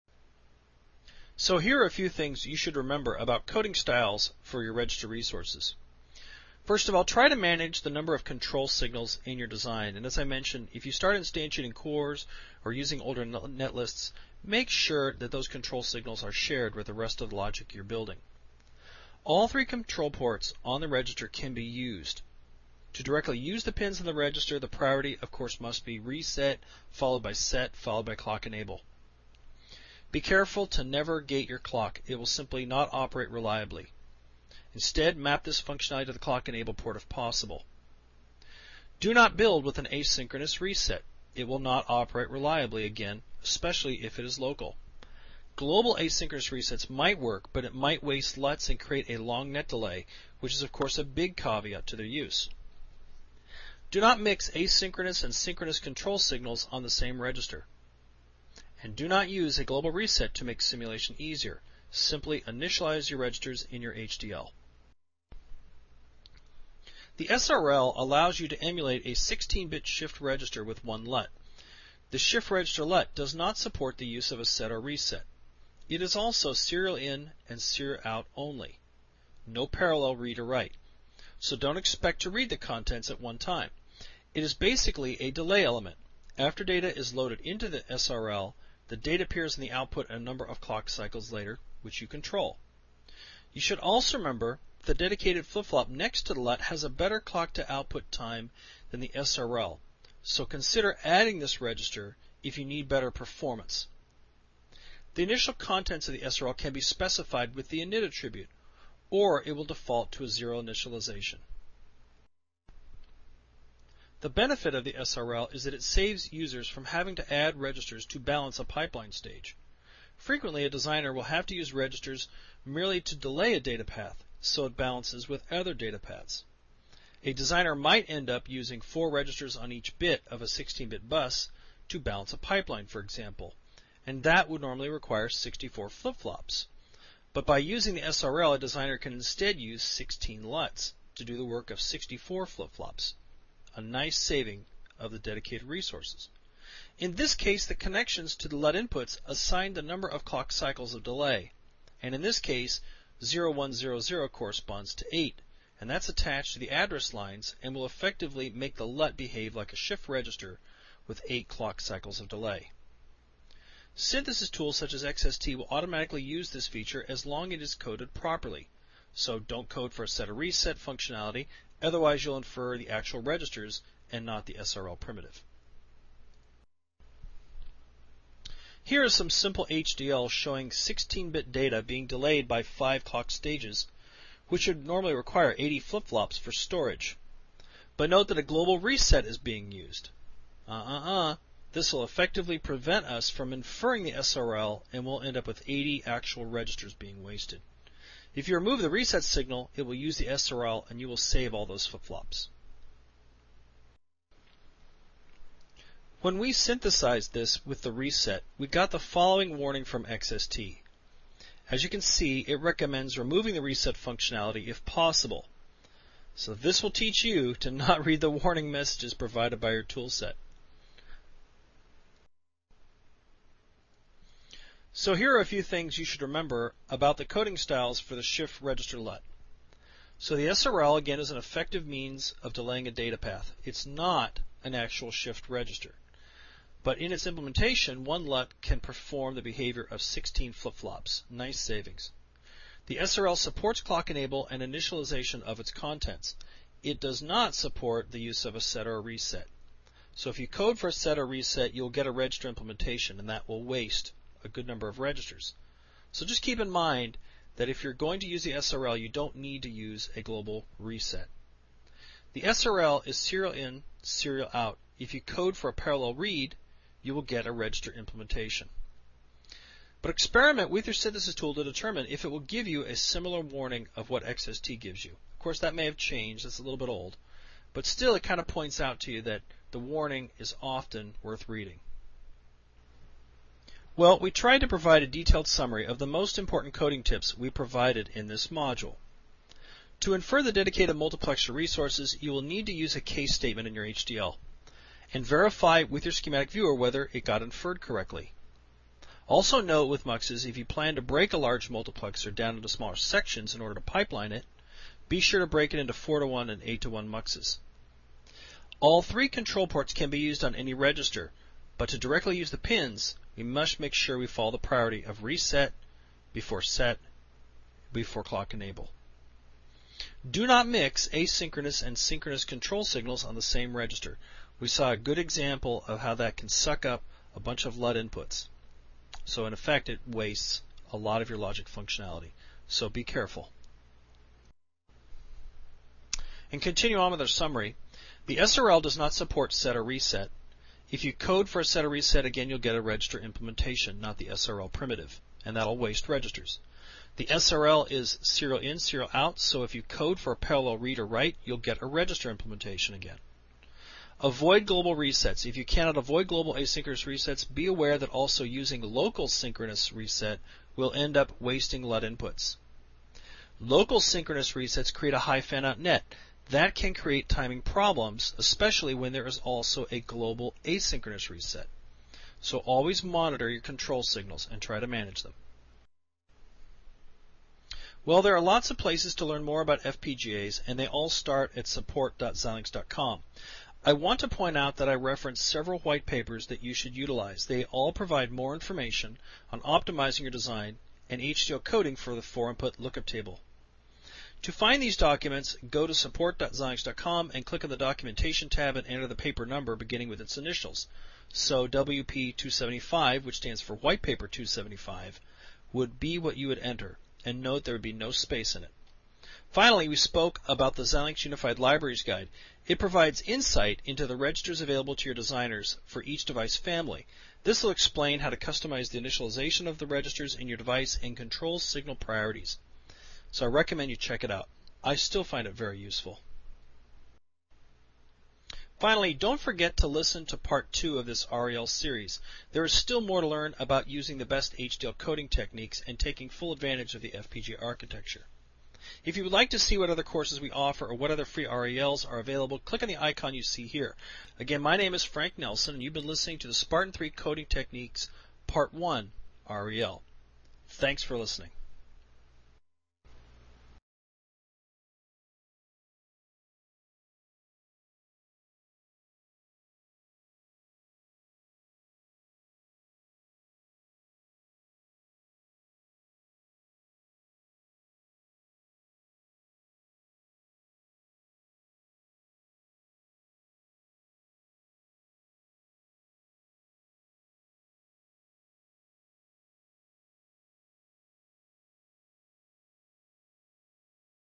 Spartan-3 FPGA HDL Coding Techniques - (Part 1, Ch 5)
How to code properly for FPGA registers, SRLs, and other dedicated resources, (for more info ) to build an efficient, high-speed FPGA design for the Spartan-3 FPGA and other 4-input, LUT-based FPGAs